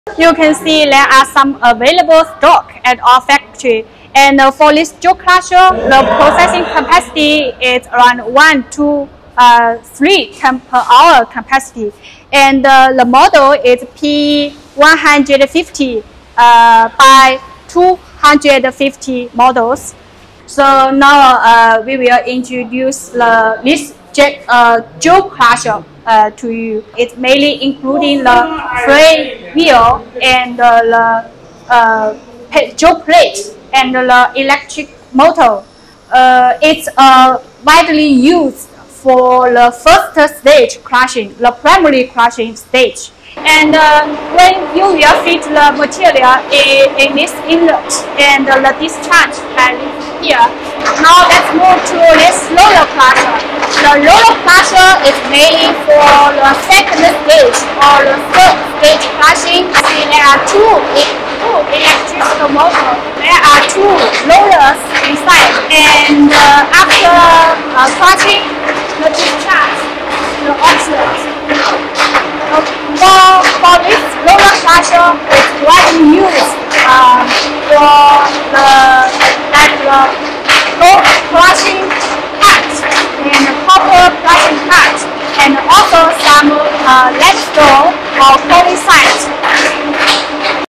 Jaw Crusher + Roller Crusher = Small Crushing Line
Jaw crushers are usually for primary crushing, and roller crushers are used in 2nd and 3rd stage.
【Contact JXSC to get details and price】